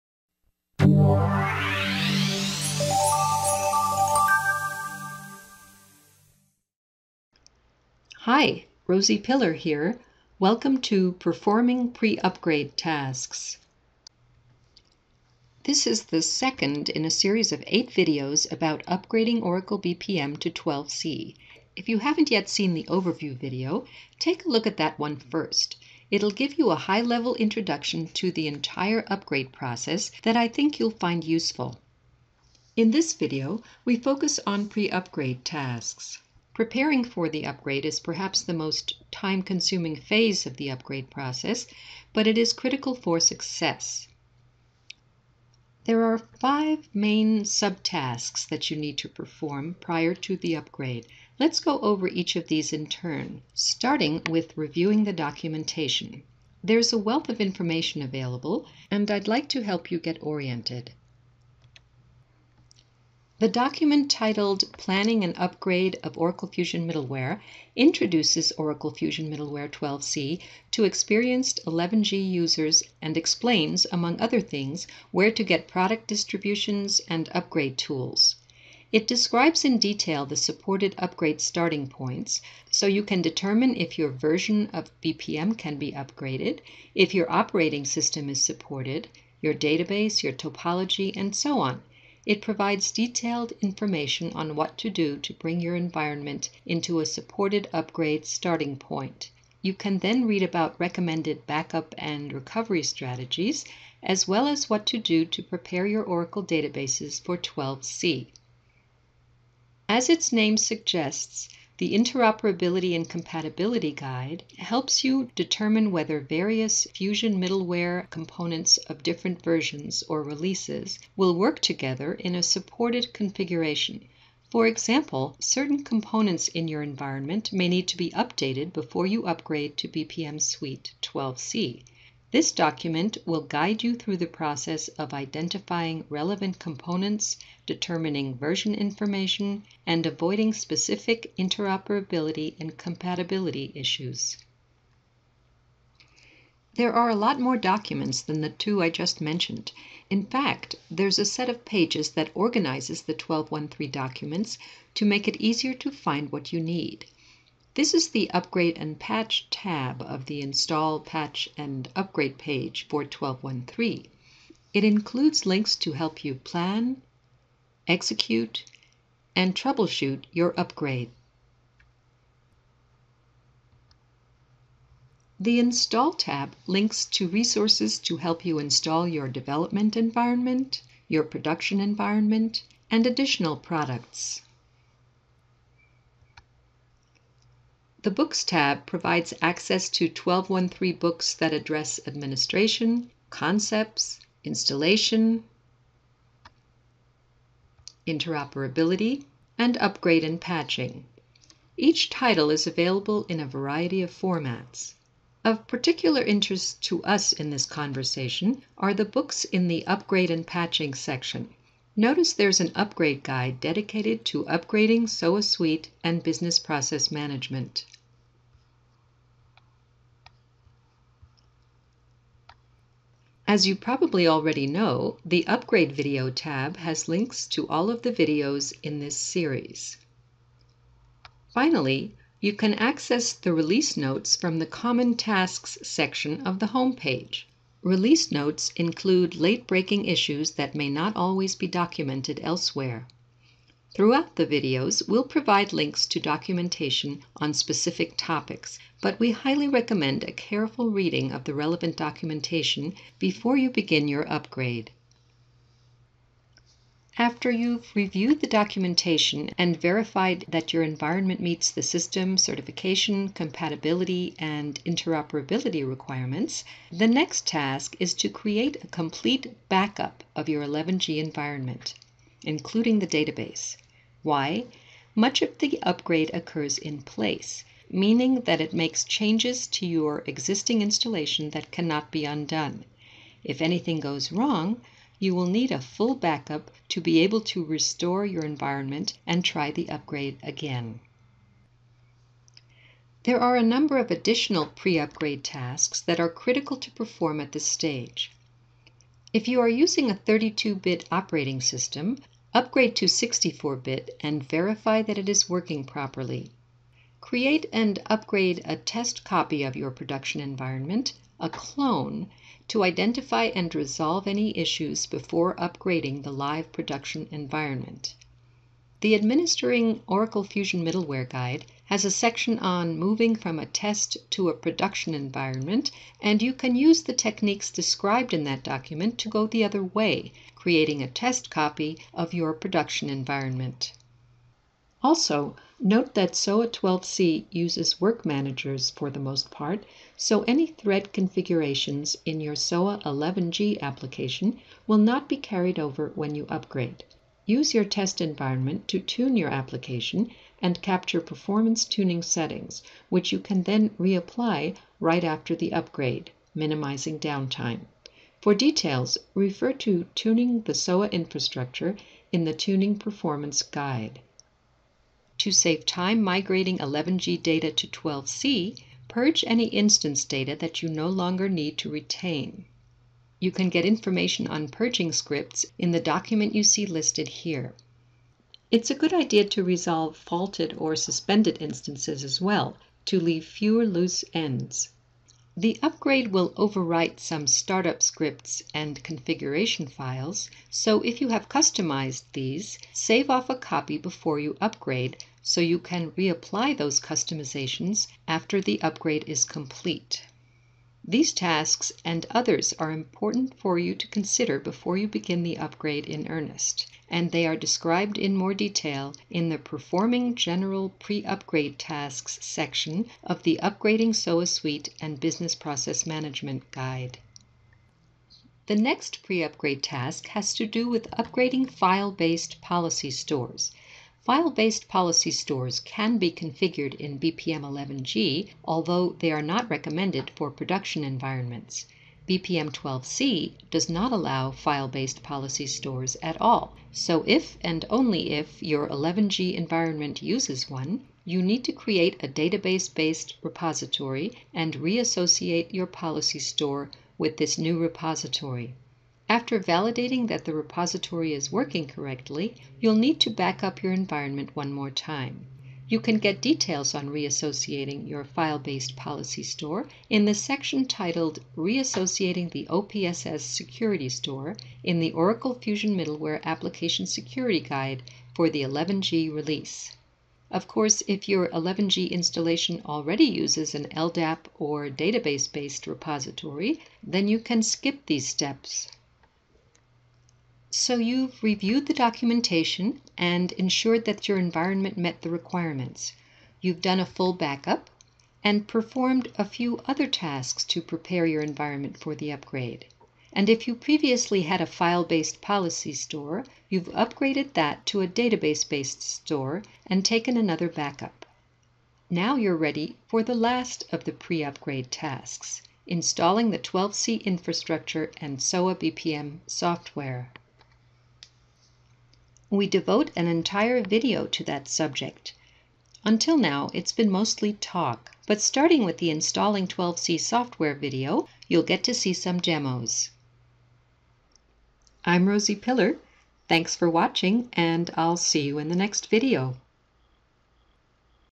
Upgrading Oracle BPM Suite to 12c, Video 2: Performing Pre-Upgrade Tasks
This is the second of eight videos about upgrading Oracle BPM Suite to 12c. It describes how to prepare for the upgrade.